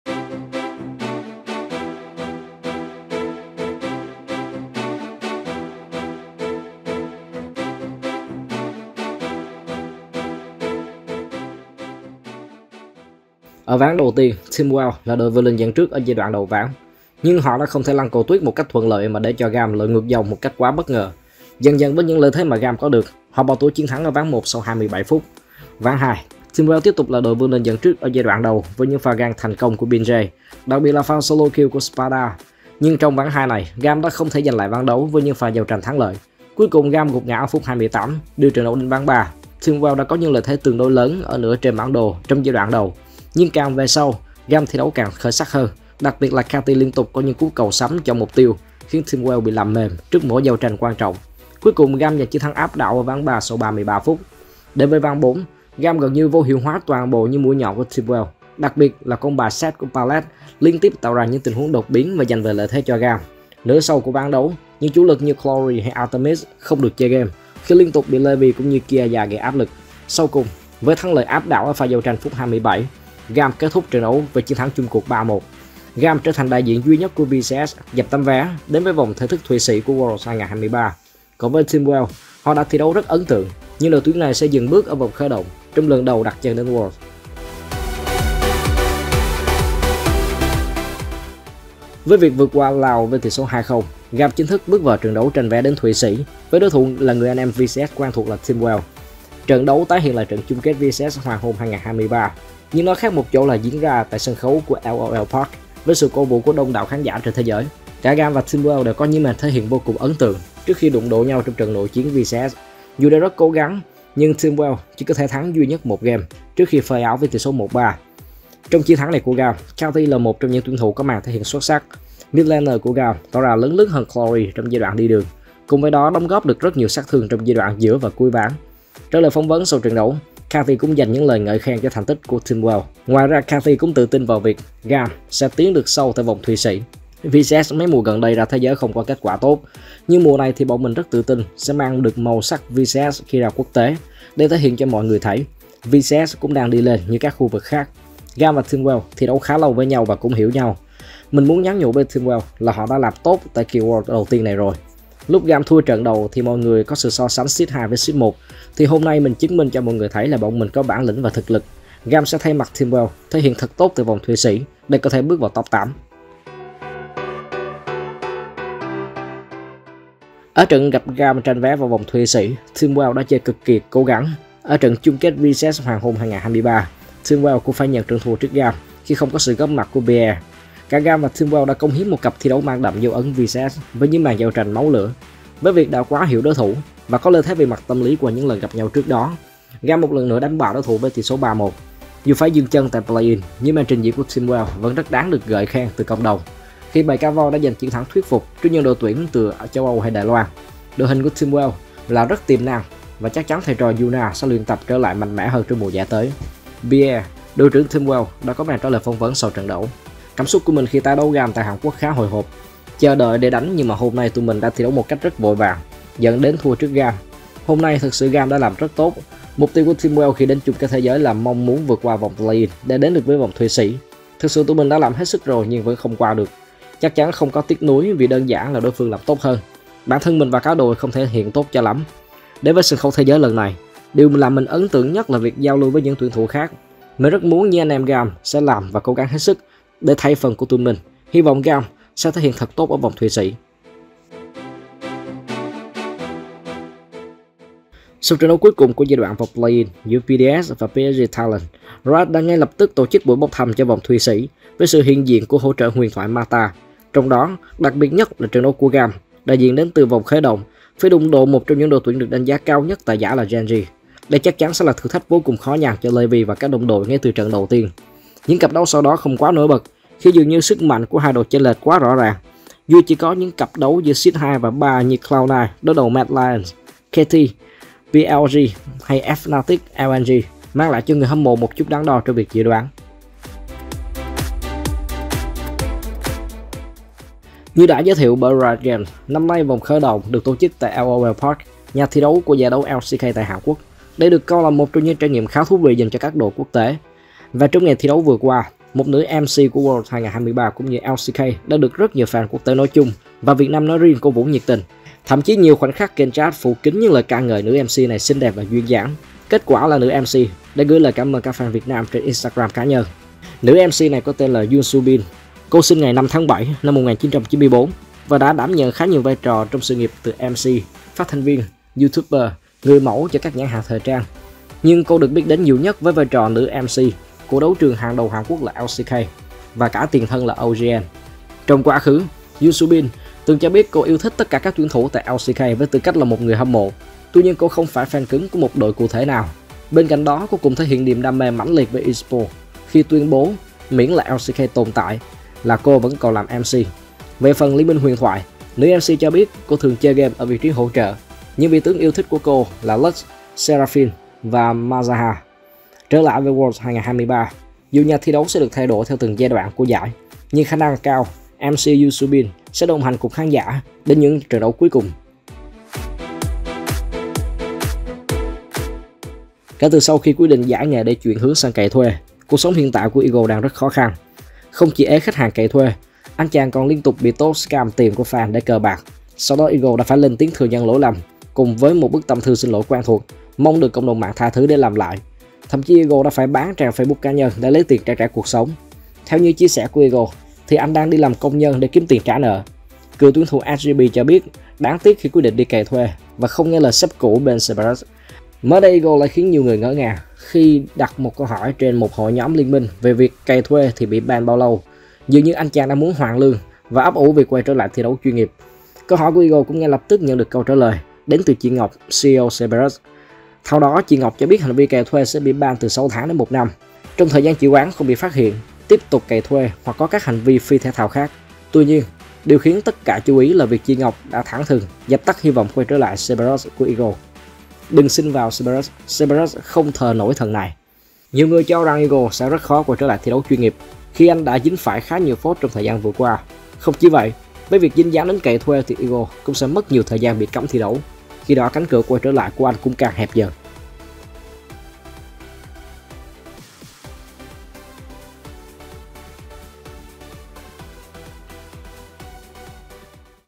BẢN TIN LMHT 16/10: ĐẢ BẠI TW - GAM VÀO VÒNG THỤY SĨ, BỐC THĂM CKTG - GAM ĐỤNG ĐỘ GEN.G
👉Ủng hộ Cảm 1 ly cafe nhé:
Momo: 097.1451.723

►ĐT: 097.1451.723